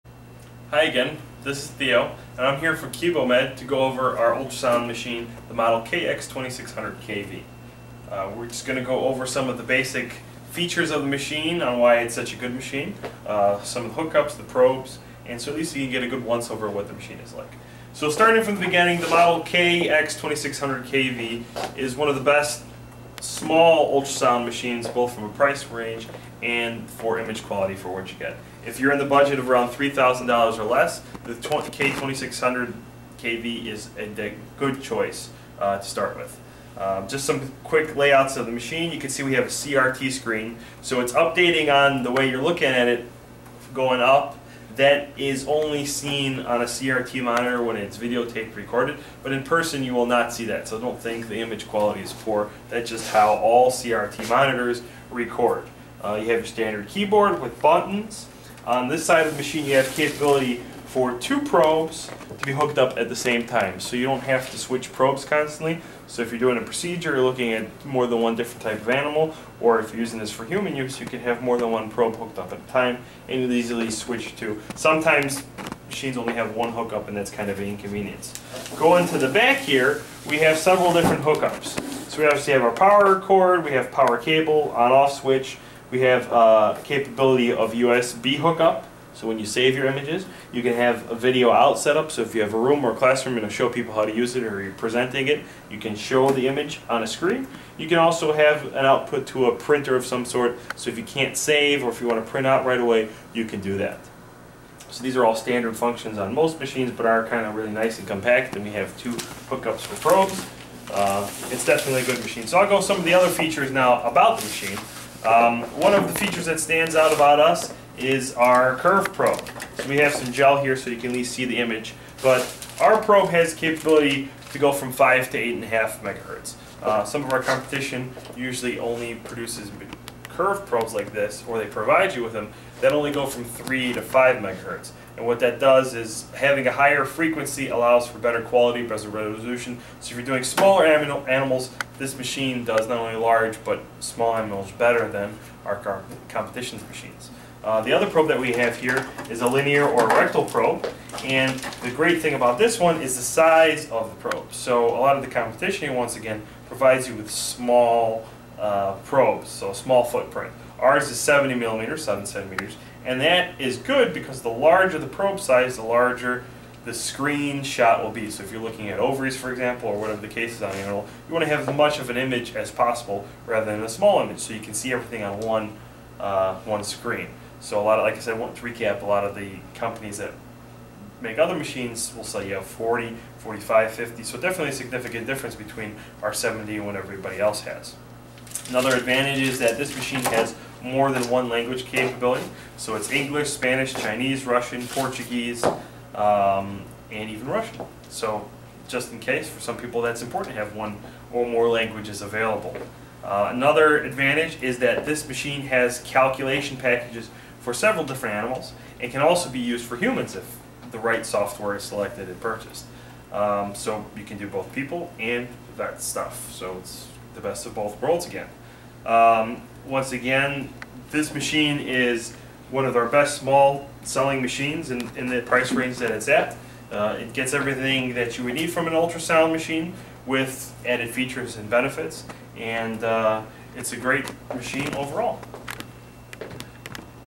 KX2600V Animal Vet Ultrasound | Basic Information
General use vet ultrasound for both small and large animals. 5-8.5MHz High Frequency Micro Convex Probe, and 5.5-7.5MHz Rectal Probe with 70mm image. Several calculations for all types of animals.